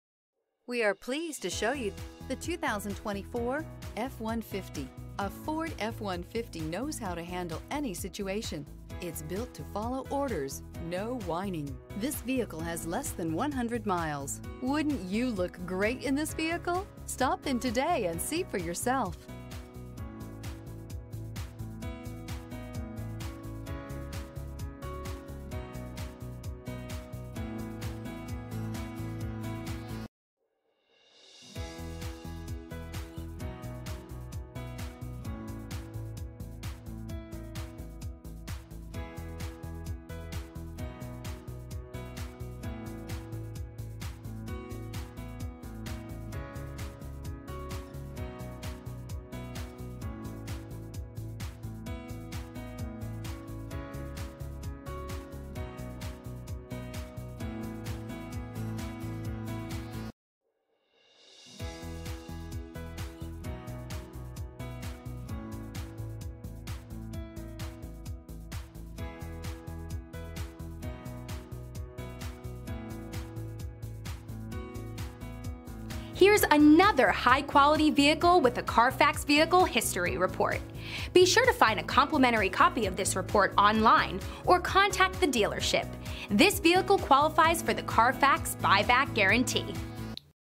2024 Ford F-150 Walk Around 00F41485
Villa Ford
2550 N. Tustin St.

Pick up this Oxford White 2024 Ford F-150, available today at Villa Ford. This could be the one you've been searching for.